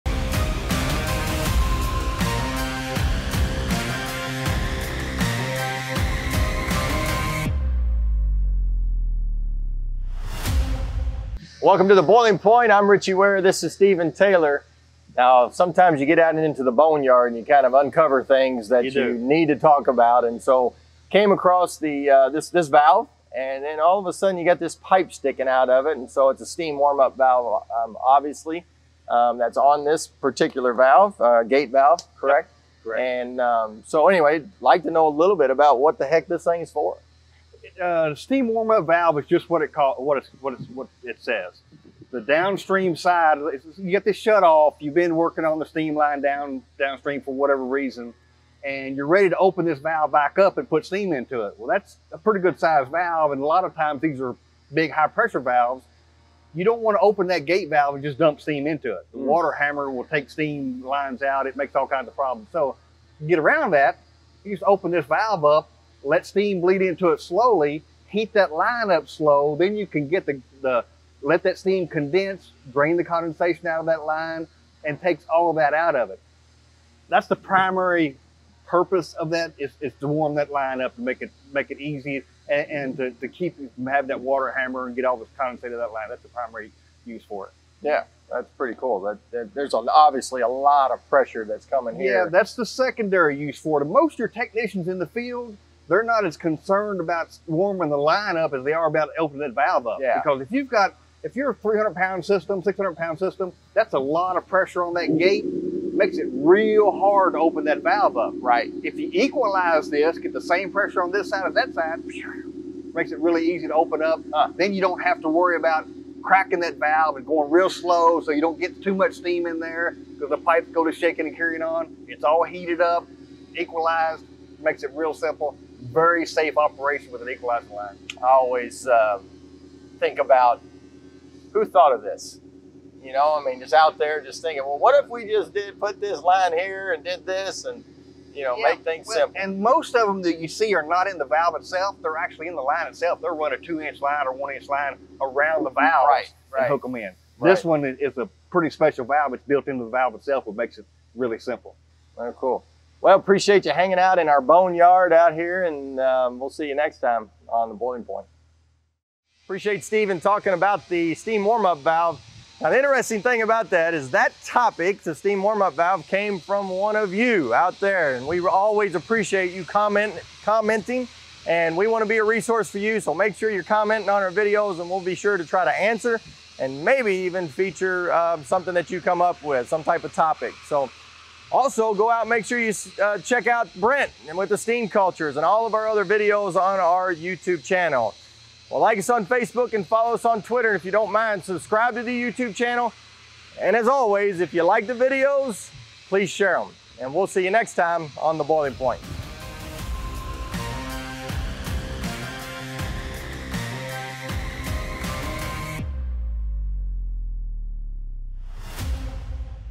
Understanding Steam Warm Up Valves in Steam Systems - Boiling Point
This episode of the Boiling Point features Ritchie in conversation with Steven Taylor about the steam warm-up valve. Learn how this essential setup facilitates smoother steam introduction into a steam system's colder regions, reducing water hammer and enhancing overall performance.

We offer nationwide boiler rental. Get a quote within an hour!